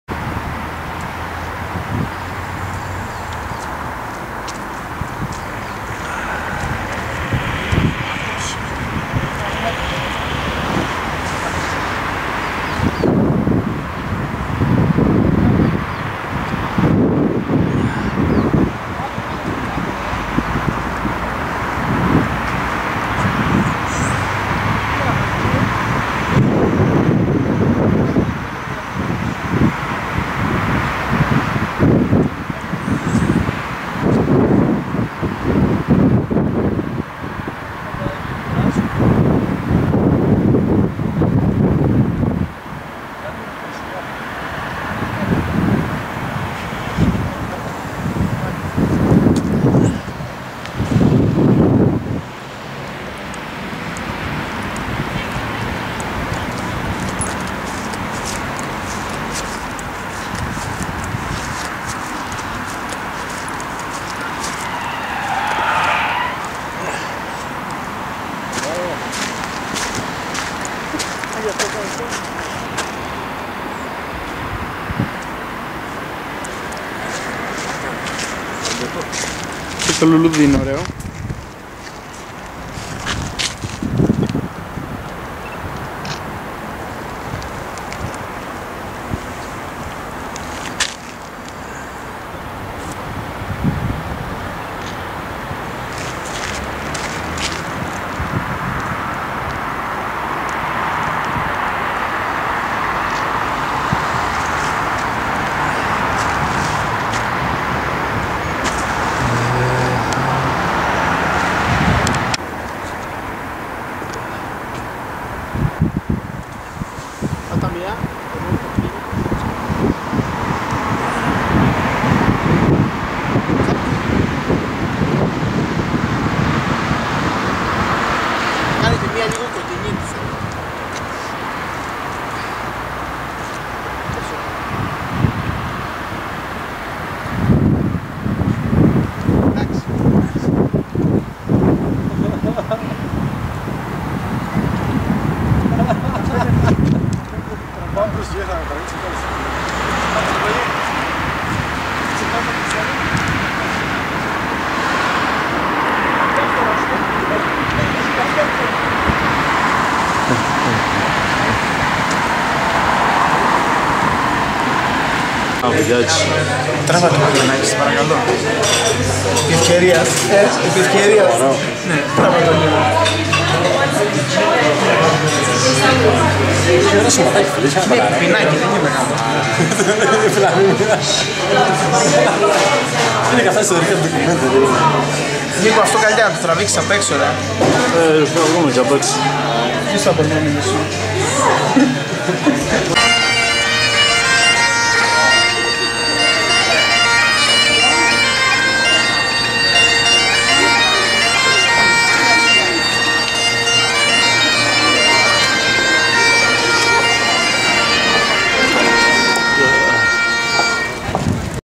Inverness castle, river Ness and Highland bagpiper
Inverness is located in the Northern part of Scotland and is considered to be the capital of the Highlands.
 It's been built around river Ness, that flows from Loch Ness into Moray Firth.
 Inverness College is situated in the city and is the main campus of the University of the Highlands and Islands, a federation of 15 colleges and research institutions in the Highlands and Islands of Scotland delivering higher education.
 Information from wiki.